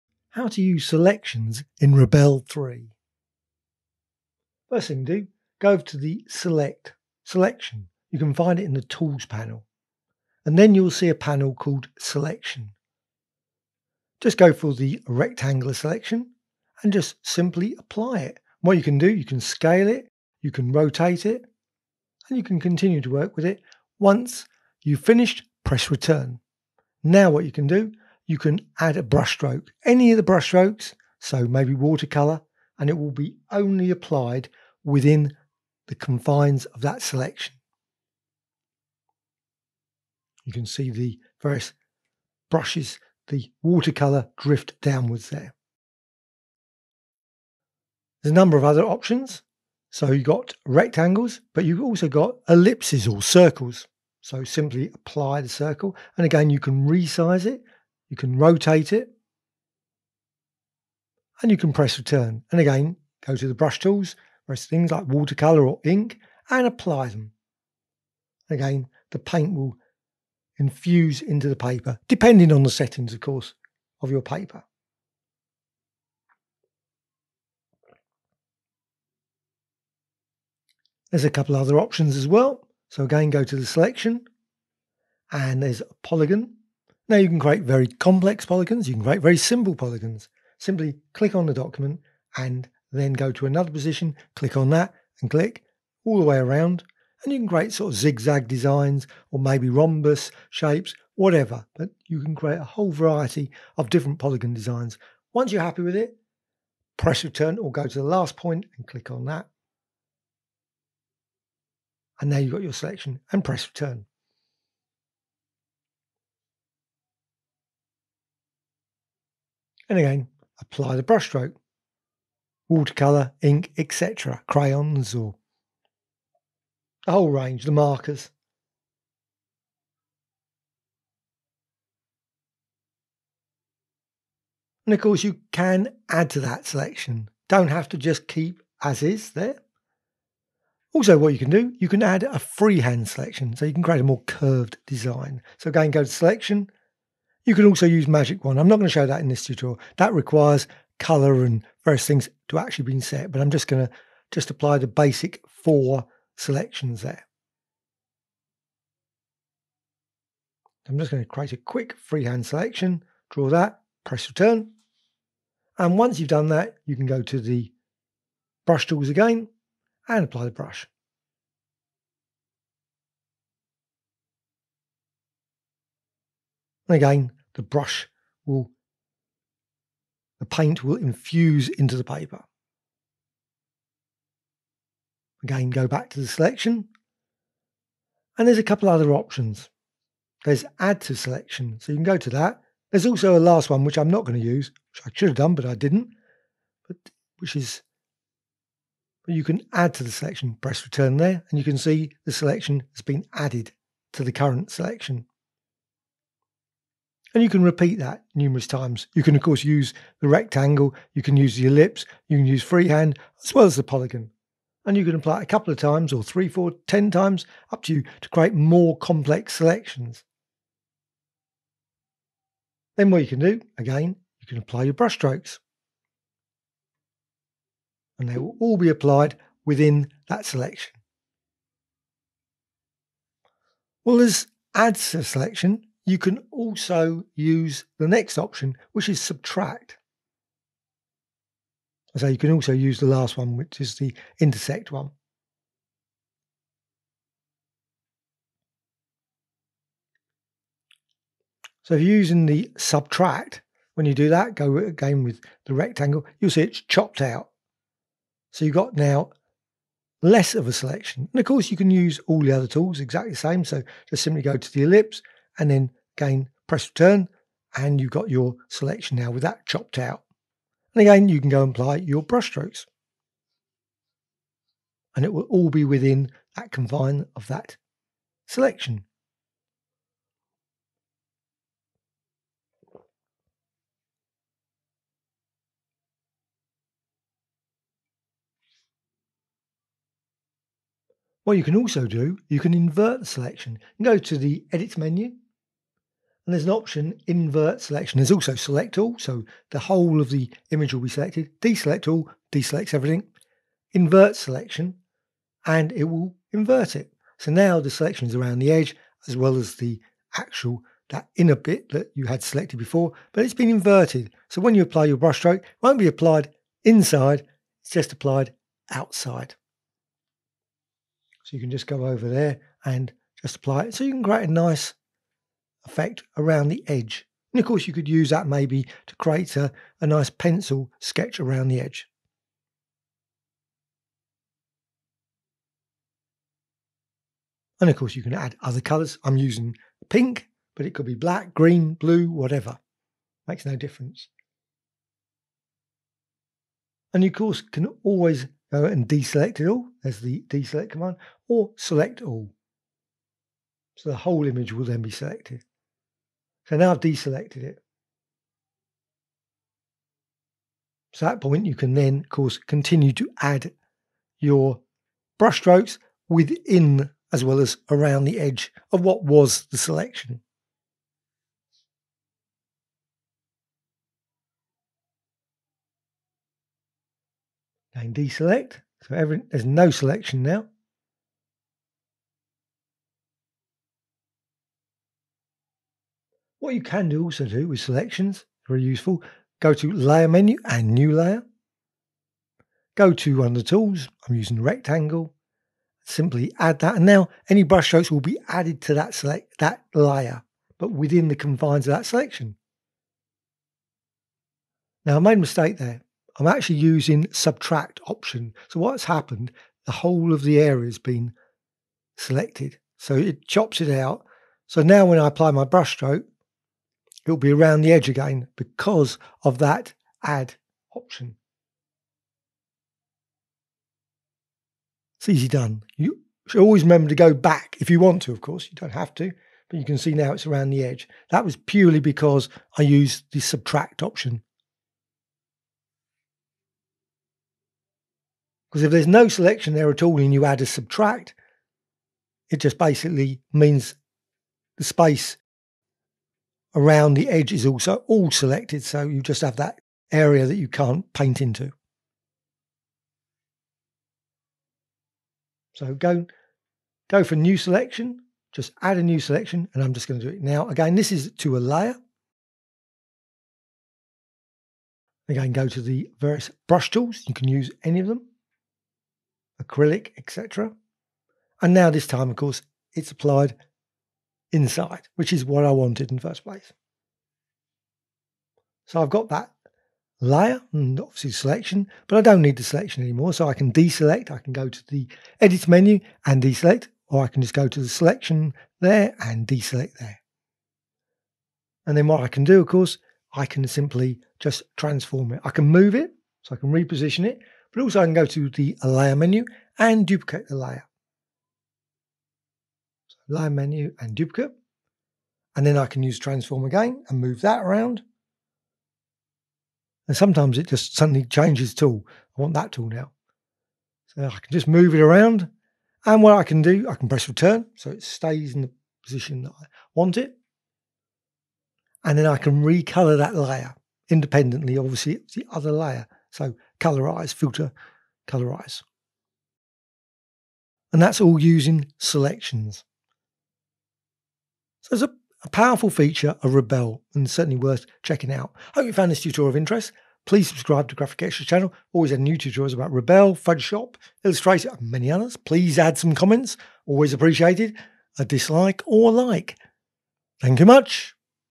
Rebelle : Selection Basics | How To Tutorial
In this video, you will learn how  to use selections in Rebelle tutorial - create all kinds of amazing selections in Rebelle and use them with your brush strokes etc

0:00 Start of tutorial on selections in Rebelle
0:10 Selection set. and rectangle
0:53 Elliptical selection
1:30 Polyon selection
2:40 Freehand selection
3:13 Add to
4:08 Subtract from
5:01 Invert selection
5:14 Apply brushes to edge
5:56 Deselect
6:28 New layer
8:22 Duplicate layer selection